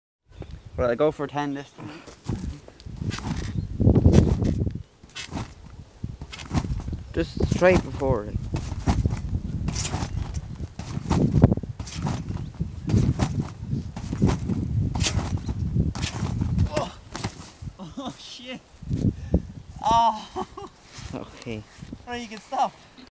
9 backflips and a fail
me trying to beet my record of 8 backflips , i get 9 but land the 9th one rong and fly off the trampoline into a dirt pile